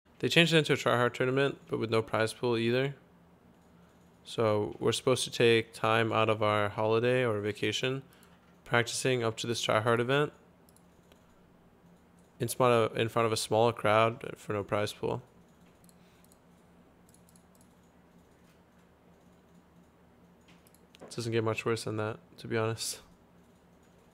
Bjergsen's thoughts on All-Stars 2017 - "It doesn't get much worse than that"
Bjergsen gives his thoughts on the League of Legends All-Stars 2017 event.